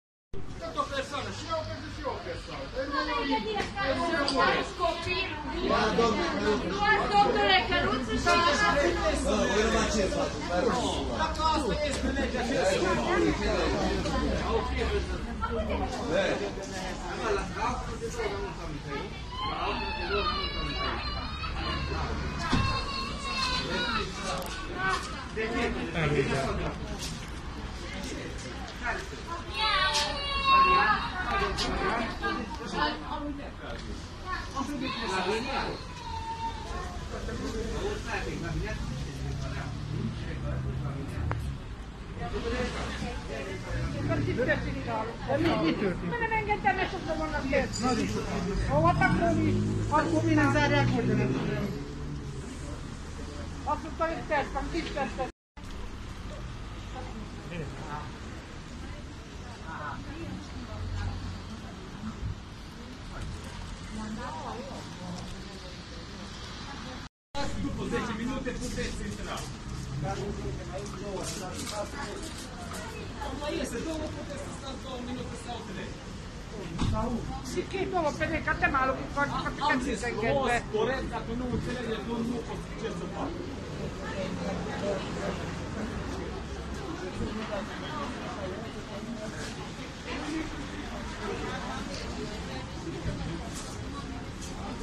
Scandal la mall ul NEPI Satu Mare Zeci de oameni tinuti la usa
Spirite încinse la intrarea la mall-ul Shopping City al celor de la NEPI Rockcastle din municipiul Satu Mare. Cozi de zeci de persoane la intrare, după ce angajații de la pază încearcă să evite pe cât posibil aglomerația din interior.